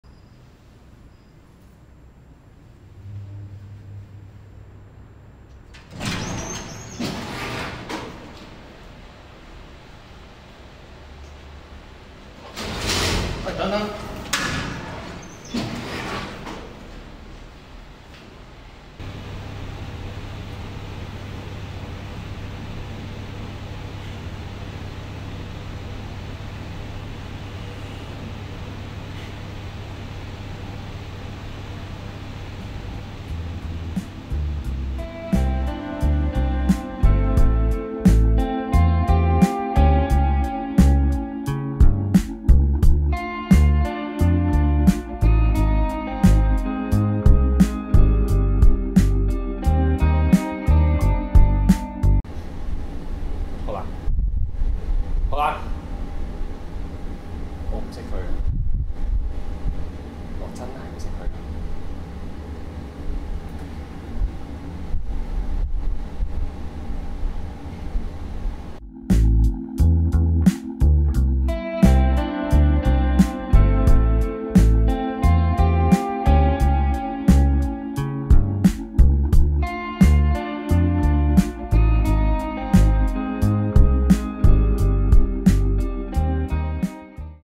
我在CoverDog的日子 02 「我唔識佢㗎⋯⋯」
Produced by CoverDog

CoverDog Presents 我在CoverDog的日子，拍下 CoverDog 的生活點滴，為大家帶來一點趣味。（其實是我們平日有太多好笑無聊東西要跟大家分享 lol）

今個星期為大家帶來 徐頴堃 Erica Chui  和 HOLAN。夢想和現實之間，隔了個馮框⋯⋯

Joe Yuen, Joann Yuen, kimlau1112, Panda Kwok, Yiet Yee Lee, Dennis Chan, DW, Felix Chan, Lau Fu Tat, Erick Shum